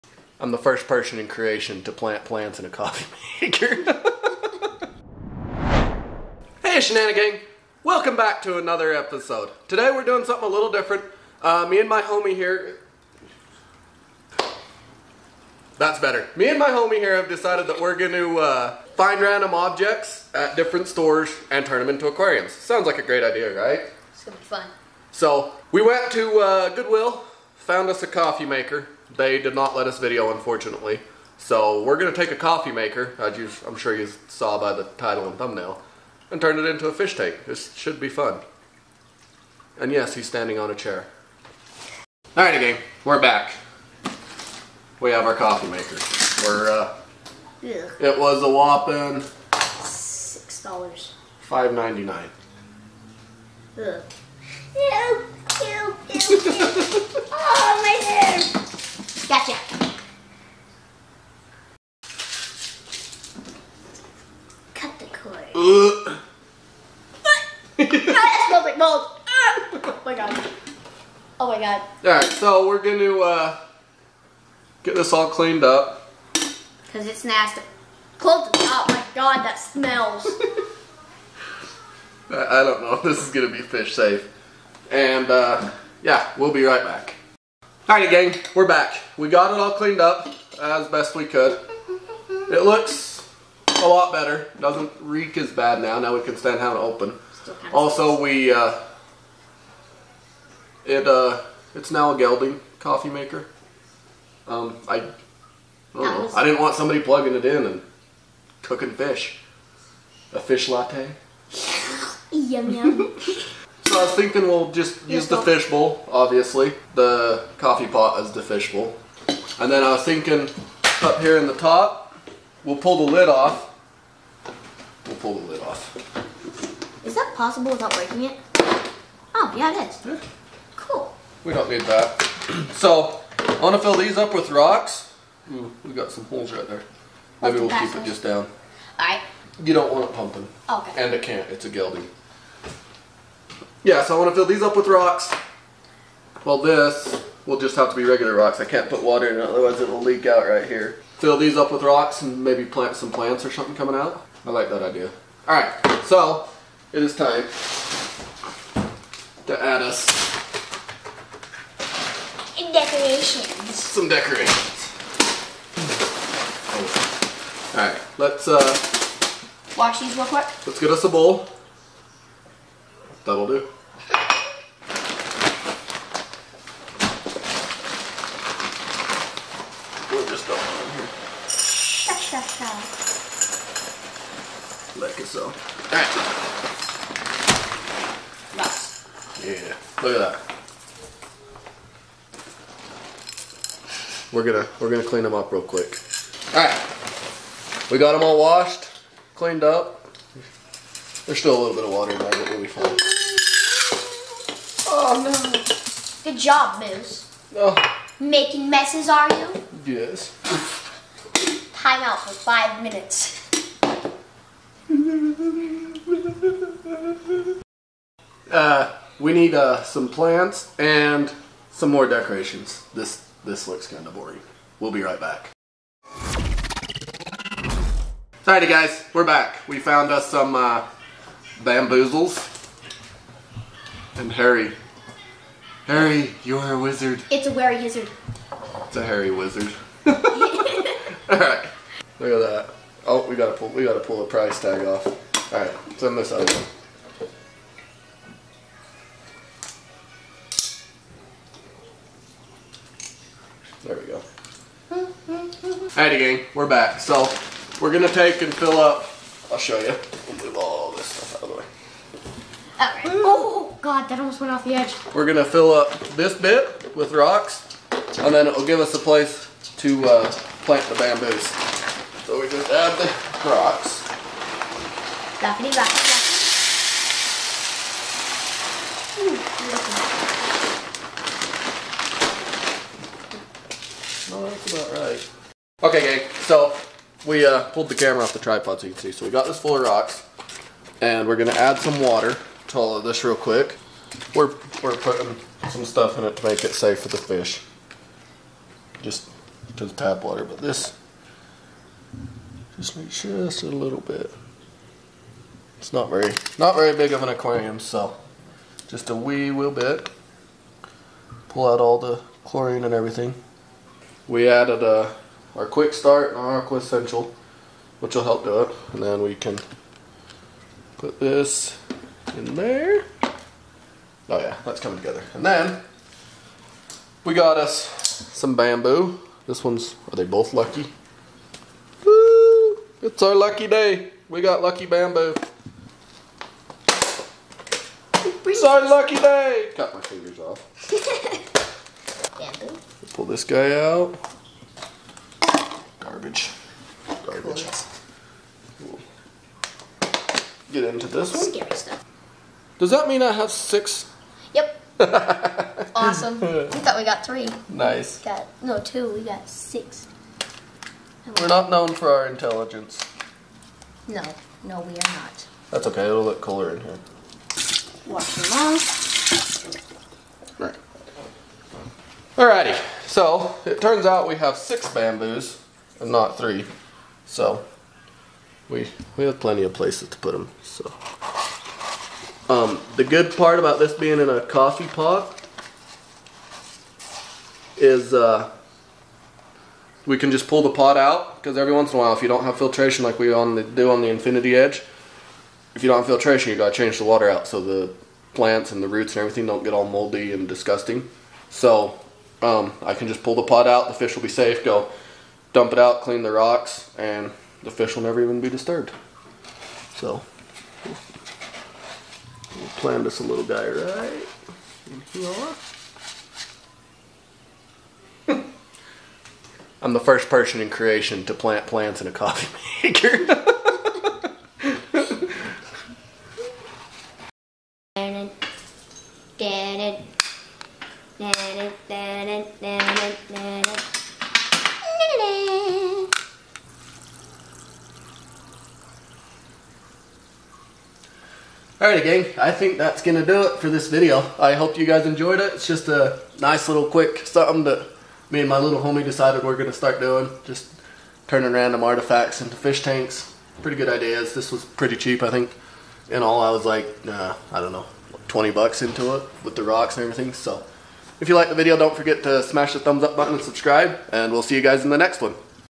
Turning a Coffee Maker into an (AQUARIUM)
Today we turn a coffee maker into an Aquarium. you defiantly dont want to miss this video
Me an my little homie start a new series of finding random stuff an turning them into an aquarium. and first on the list is a coffee maker
Tiktok Shenanigan-hub